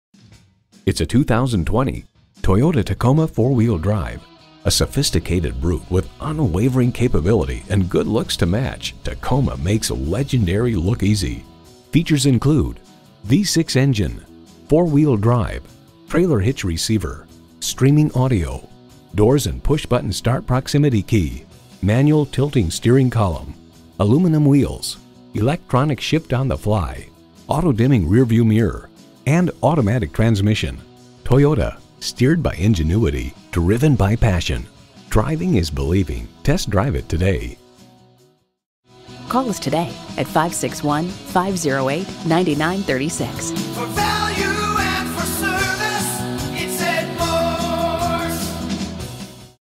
Used 2020 Toyota Tacoma West Palm Beach Juno, FL PE007861A - SOLD
Year: 2020
Make: Toyota
Model: Tacoma
Trim: SR5 V6
Engine: 3.5Ll 6 Cylinder Fuel Injected
Transmission: 4WD 6-Speed Automatic
Interior: Cement
Mileage: 27064
Stock #: PE007861A
VIN: 3TYCZ5AN0LT003464
Recent Arrival! 2020 Toyota Tacoma SR5 V6 Gray ED MORSE LIFETIME POWERTRAIN WARRANTY *See Dealer for Details* . Carfax Accident Free, Local Trade, 4WD. Odometer is 12757 miles below market average! 4WD, 6 Speakers, ABS brakes, Air Conditioning, Alloy wheels, AM/FM radio: SiriusXM, Anti-whiplash front head restraints, Apple CarPlay/Android Auto, Auto High-beam Headlights, Auto-dimming Rear-View mirror, Axle Ratio: 3.91, Brake assist, Bumpers: body-color, Color-Keyed Overfenders, Daytime Running Lamps, Driver door bin, Dual front impact airbags, Dual front side impact airbags, Electronic Stability Control, Emergency communication system: Safety Connect with 1-year trial, Exterior Parking Camera Rear, Fabric Seat Trim (FC), Front anti-roll bar, Front Bucket Seats, Front Center Armrest, Front Fog Lamps, Front fog lights, Front reading lights, Front wheel independent suspension, Illuminated entry, Knee airbag, Low tire pressure warning, Occupant sensing airbag, Outside temperature display, Overhead airbag, Overhead console, Panic alarm, Passenger door bin, Power door mirrors, Power driver seat, Power Sliding Rear Window w/Privacy Glass, Power steering, Power windows, Radio: AM/FM/HD Radio, Rear step bumper, Remote keyless entry, Speed control, Speed-sensing steering, Split folding rear seat, Steering wheel mounted audio controls, Tachometer, Telescoping steering wheel, Tilt steering wheel, Traction control, Trip computer, Variably intermittent wipers, and Wheels: 16 x 7J Silver Alloy.

Address:
3790 West Blue Heron Boulevard